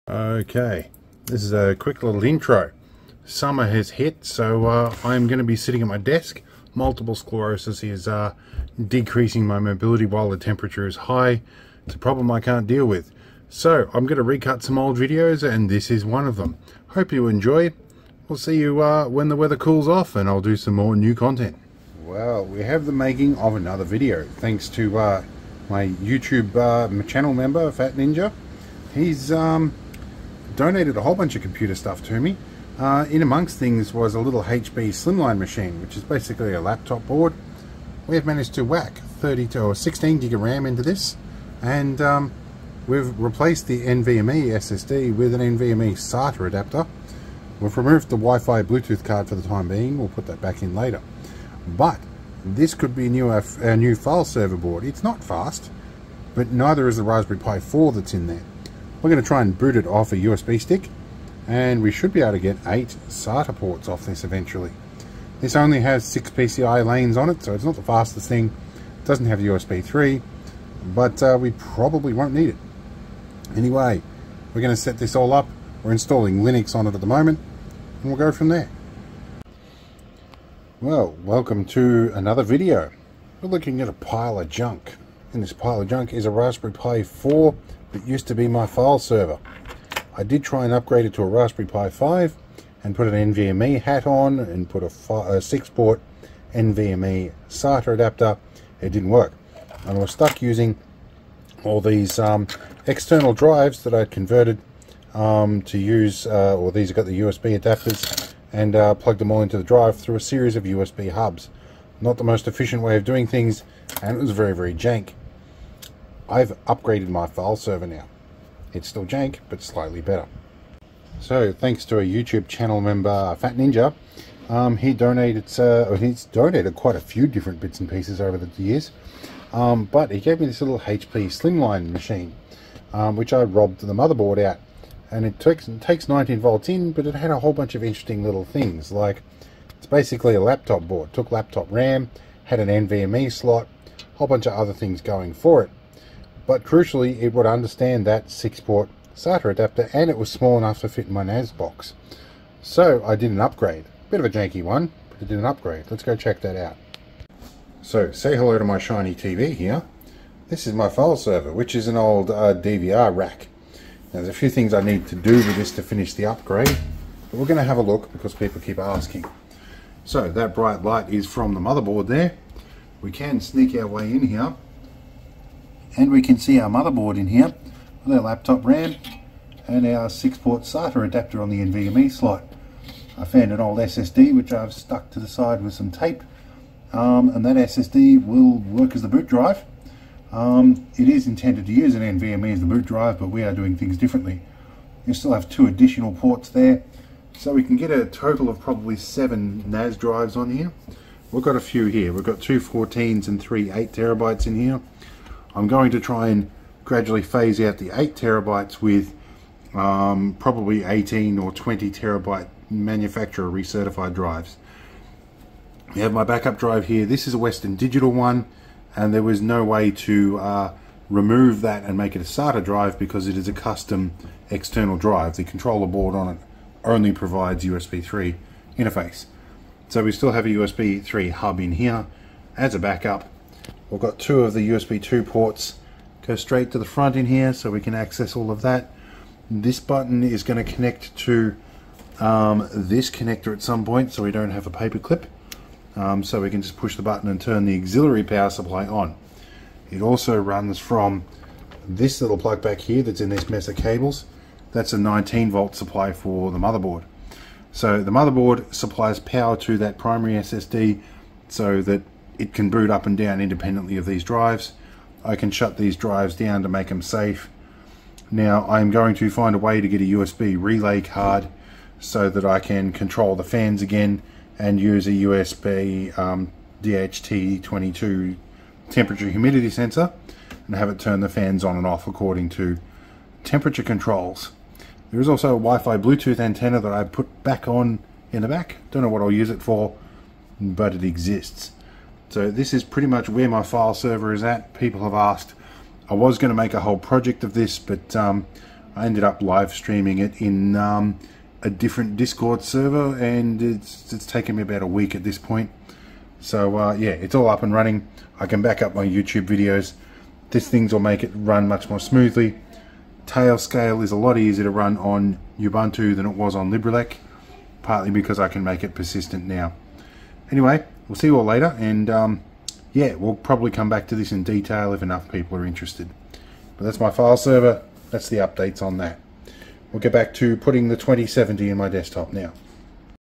No more Raspberries in my file server! - Personal NAS "Upgrade"
I finally replaced the Raspberry pPi 4 in my file server, after the RPI5 did not like the NVME SATA card.